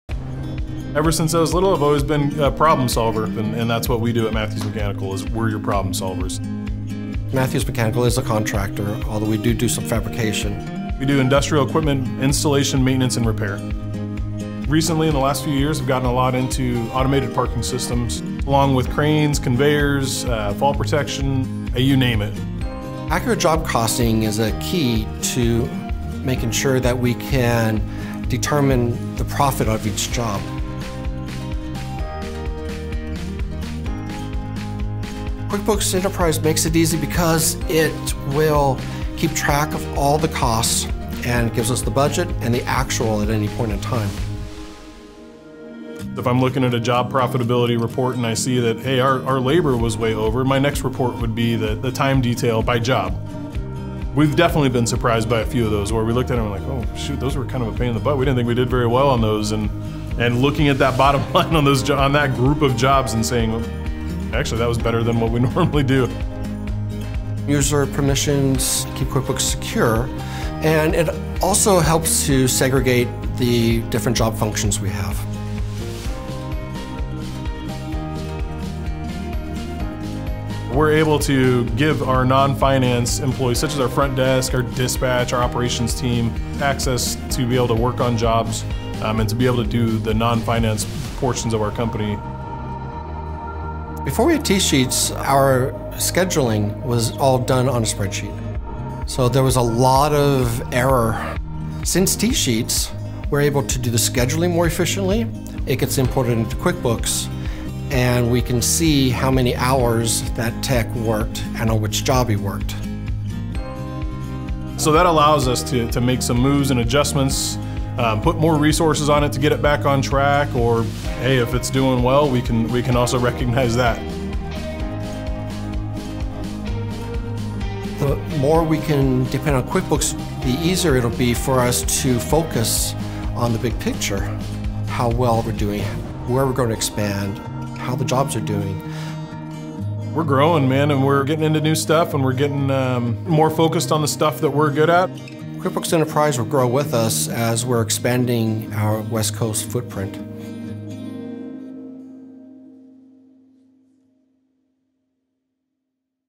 Tracking profits with accurate job costing | QuickBooks Enterprise
See how QuickBooks Enterprise works through the eyes of industrial contractor Mathews Mechanical. (Learn more Job profitability reports let you see if your labor is over or under estimates so you keep your profits on track. Advanced user permissions keep your data secure and help you segregate the different job functions you have between your staff. TSheets time tracking helps you reduce errors and save time as all your time tracking data is rolled up into QuickBooks. Now you can schedule work more efficiently. You also can expand Enterprise with more than 200 different apps. Enterprise will grow with you just as it grows with Mathews Mechanical.

In this video:
0:00 Mathews Mechanical is an industrial equipment, installation, maintenance, and repair contractor.
0:23 Accurate job costing is a key to making sure that they can determine the profit of each job.
0:38 QuickBooks Enterprise makes it easy to accurately track job costs.
0:41 Enterprise will keep track of all the costs and it gives Mathews the budget and the actual costs at any time.
0:47 Job profitability reports let you see if your labor is over or under estimates.
1:05 Advanced user permissions keep your data secure and help you segregate the different job functions you have.
1:22 Mathews can give non-finance staff - like the front desk, dispatch, and the operations team - access to work on jobs in the non-finance portions of the company.
1:38 Before TSheets time tracking, scheduling was all done in a spreadsheet so there were a lot of errors.
1:42 Since they got TSheets with Enterprise, they’re able to do the scheduling more efficiently, it gets imported into QuickBooks, and they can see how many hours a tech worked on which job.
2:00 All the tracking lets them make adjustments on a job as needed to keep them more profitable.
2:07 Enterprise lets you connect to over 200 apps to automate and streamline your workflows.
2:15 With QuickBooks, Mathews is able to expand and focus on the big picture.
2:20 QuickBooks Enterprise will grow with them as Mathews Mechanical continues to expand its West Coast footprint.